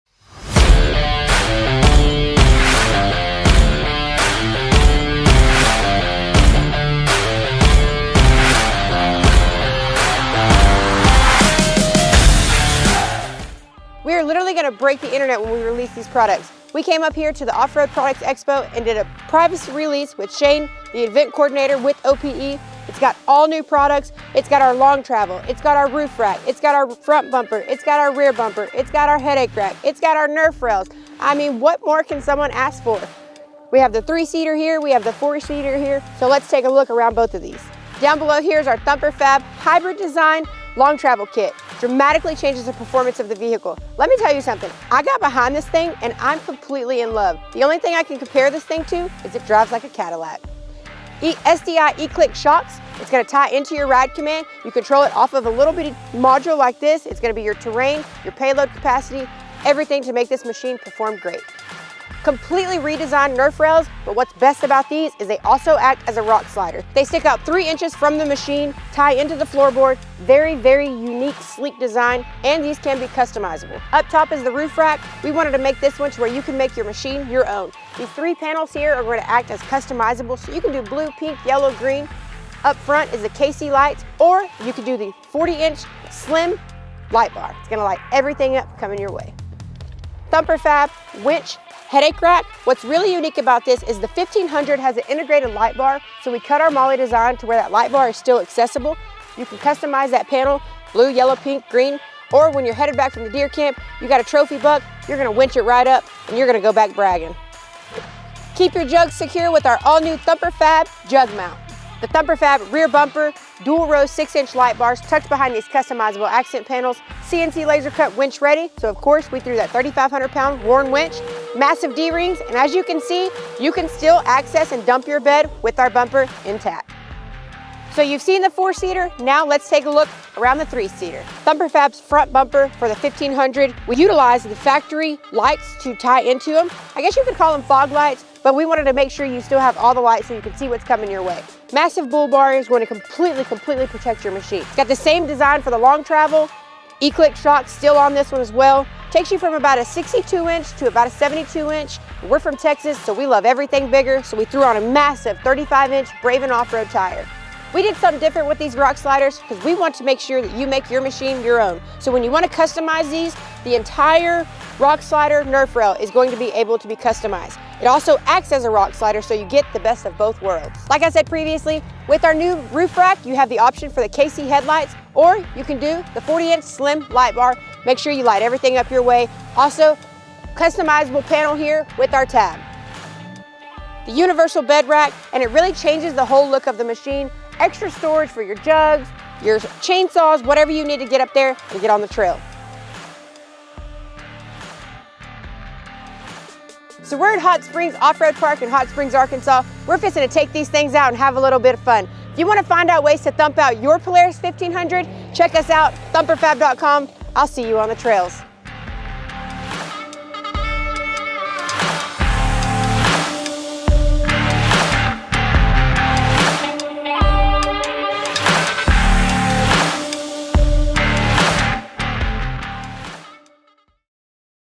The BEST Accessories for Your 2024 Polaris Ranger 1500 XD | Thumper Fab
🛠️ Gear up your 2024 Polaris Ranger 1500 XD with the best accessories from Thumper Fab! In this exciting video, we unveil a curated selection of top-quality accessories designed to elevate the performance, functionality, and style of your off-road beast. Whether you're seeking enhanced protection, increased storage capacity, or aggressive aesthetics, Thumper Fab has the perfect accessories to customize your Polaris Ranger 1500 XD according to your needs and preferences.

Join us as we showcase the must-have accessories for your 2024 Polaris Ranger 1500 XD, including:
🔧 Front and rear bumpers engineered for maximum durability and protection, ensuring your Ranger 1500 XD is ready to tackle any terrain with confidence.
🌟 Roof racks, cargo boxes, and storage solutions to expand your hauling capabilities and carry all your gear for extended adventures.
💡 LED light bars, pod lights, and lighting kits for improved visibility and safety during night rides or low-light conditions.
💪 Suspension upgrades, bumpers, and other performance-enhancing accessories to take your off-road experience to the next level.
🚙 Stylish accents, trim pieces, and decals to personalize your Polaris Ranger 1500 XD and make it stand out on the trails or at the campsite.

With Thumper Fab's best accessories for the 2024 Polaris Ranger 1500 XD, you can customize your UTV to match your unique lifestyle and off-road demands.

Join us on this accessory showcase, and discover how you can transform your 2024 Polaris Ranger 1500 XD into the ultimate off-road machine with Thumper Fab!

Be sure to check out more 2024 Polaris Ranger 1500 accessories

Ready to enhance your 2024 Polaris Ranger 1500 XD? Let's explore the best accessories from Thumper Fab! 🚙💨